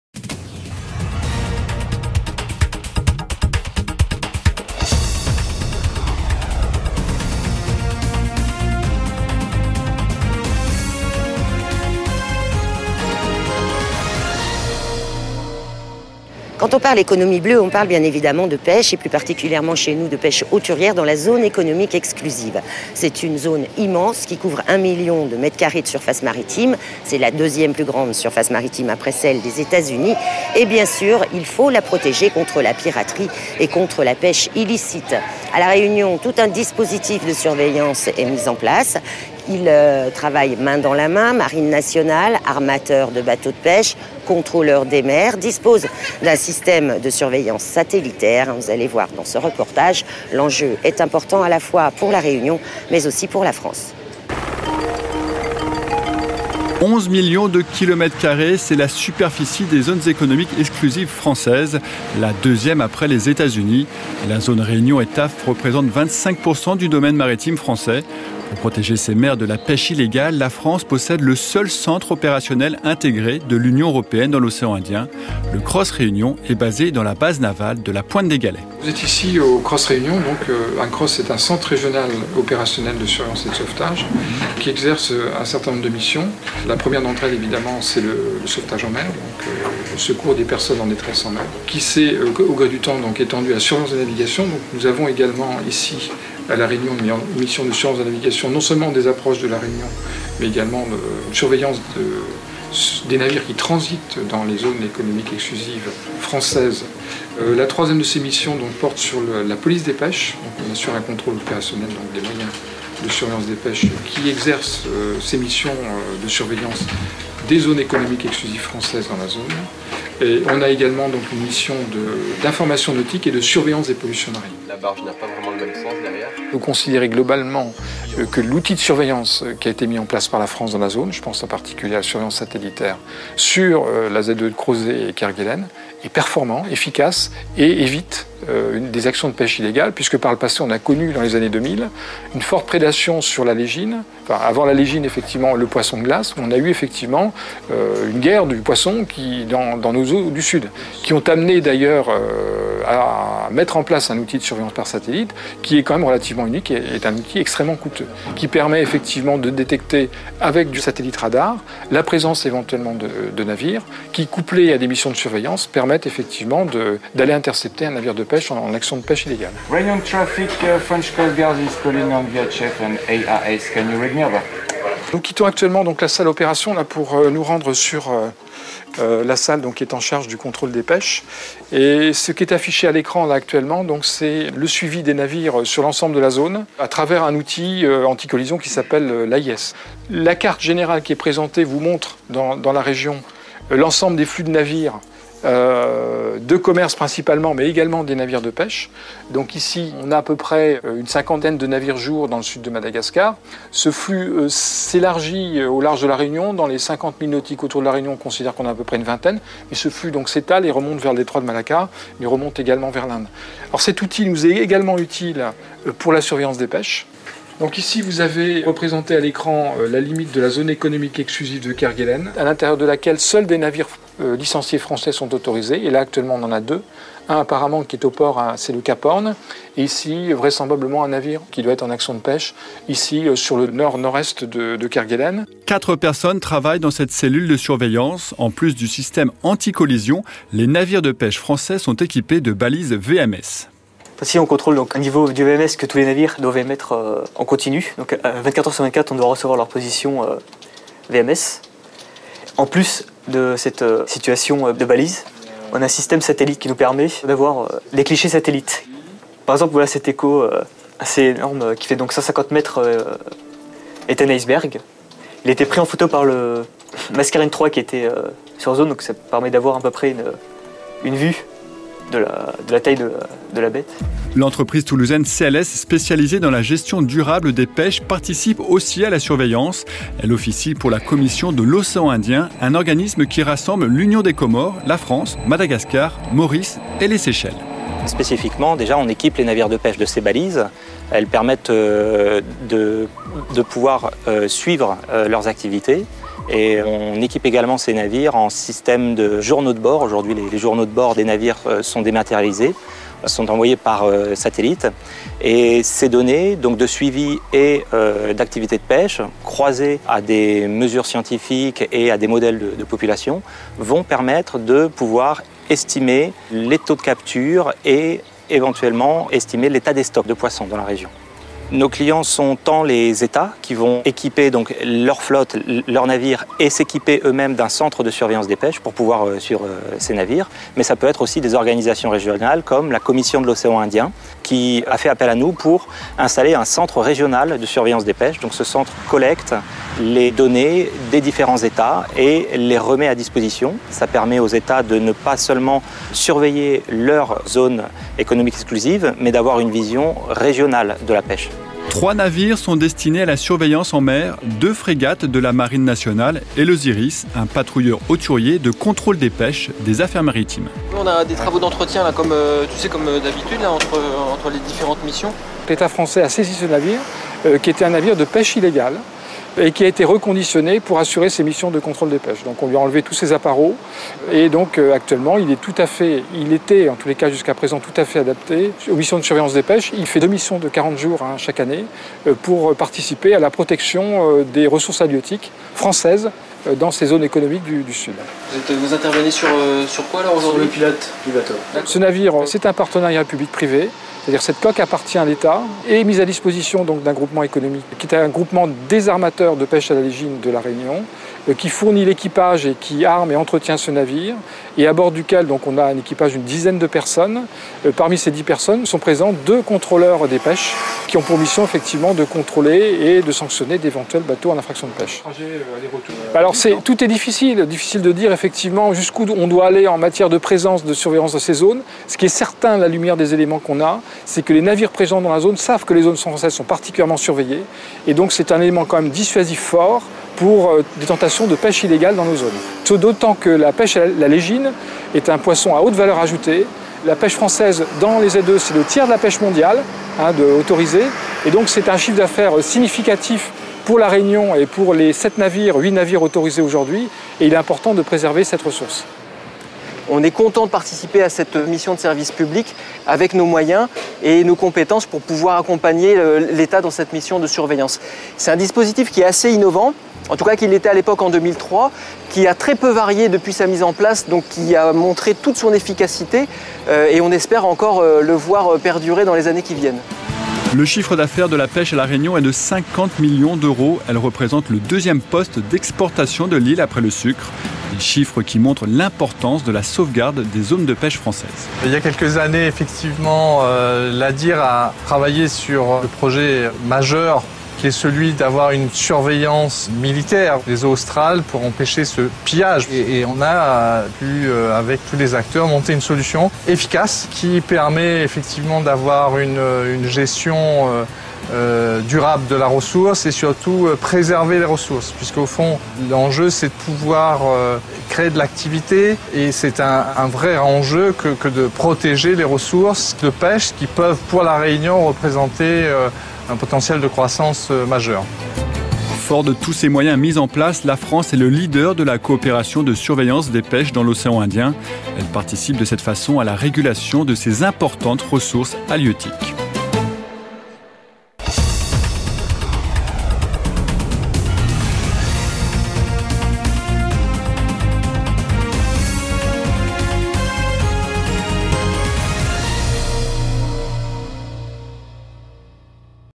Lutte contre la piraterie et la pêche illicite (juin 2017)
La lutte contre la piraterie et la pêche illicite est un enjeu majeur pour La Réunion et implique une surveillance de la Zone Economique Exclusive.
(copyright ADIR, juin 2017)